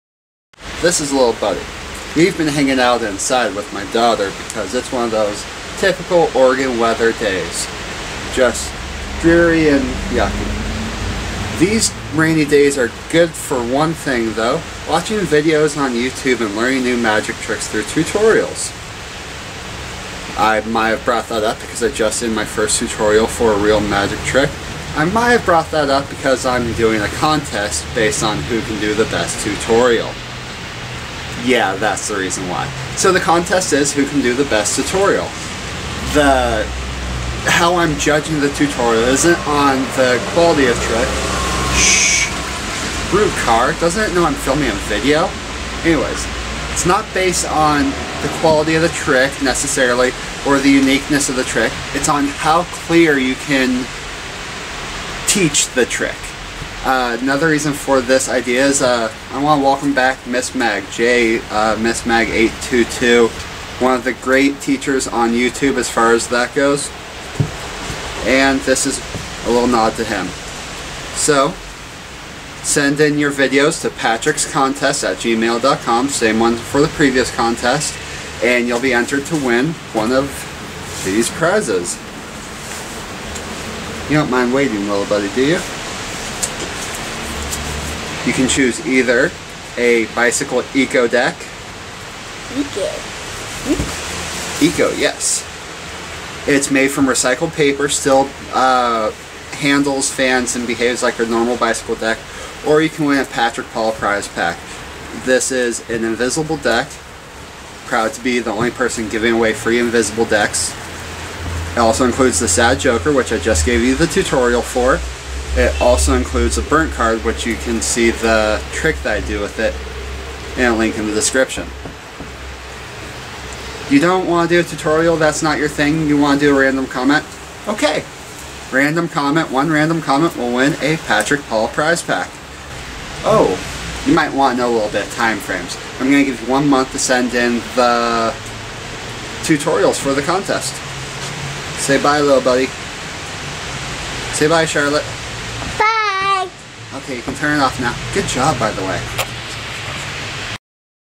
Tutorial Contest and Random Giveaway
There is a limit of three entries per Person, but no time limit on the entries.
For the random draw you can enter one comment per day for 30 days.
Contest will end Feb 11th, My Beautiful Wifes Birthday